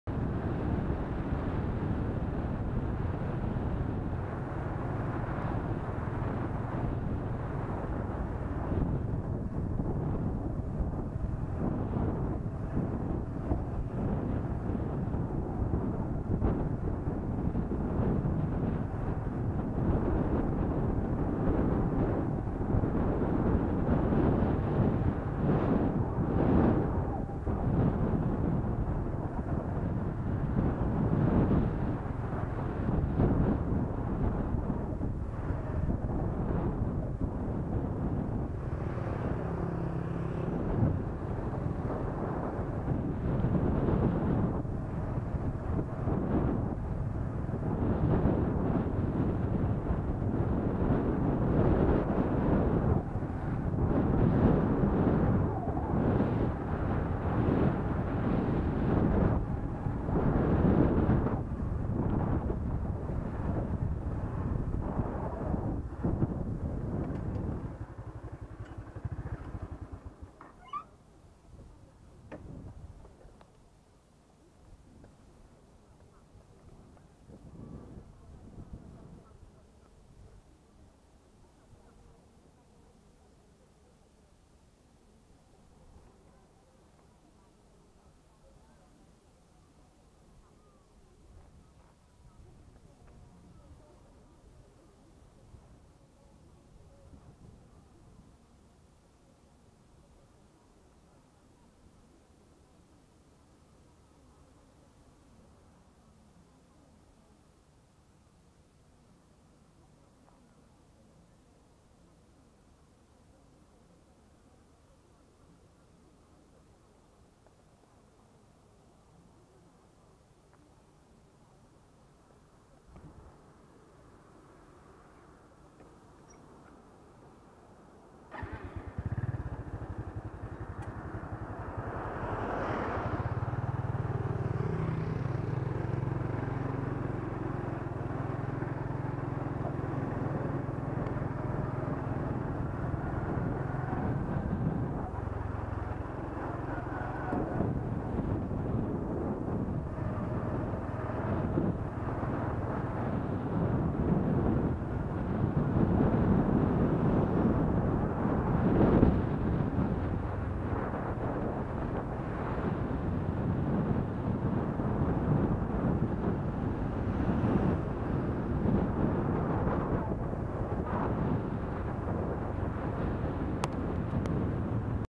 Cầu Đồng Quang TL 87A bắc qua sông Đà
Chúng thủy giai đông tẩu
Đà giang độc bắc lưu
Cây cầu ngay chân núi Ba Vì, có thể vừa ngắm sông vừa ngắm núi Ba Vì khá cao nơi đồng bằng.